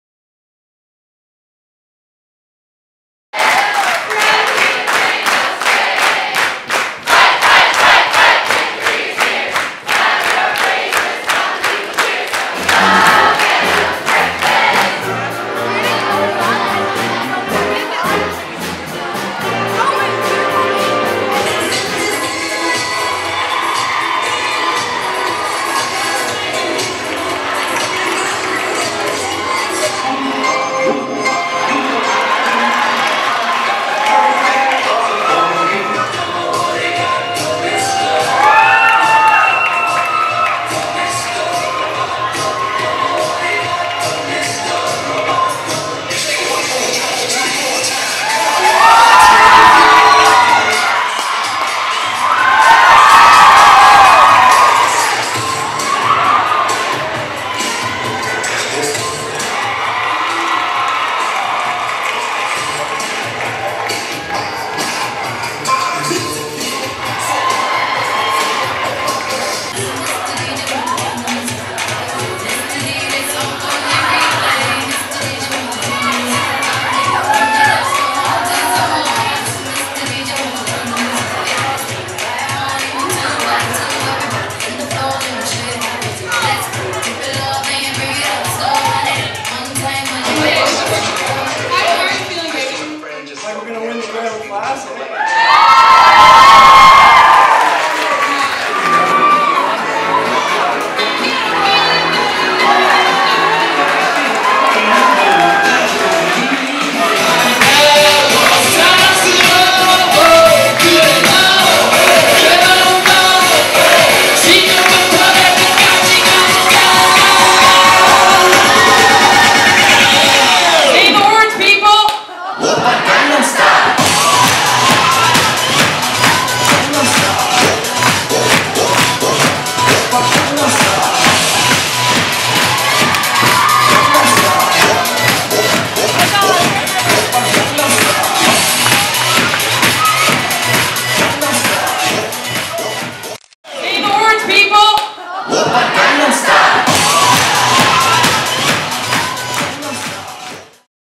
Grammar Classic Pep Rally
Pep rally before the 7th annual Grammar Classic, an invitational basketball tournament hosted by Halifax Grammar School.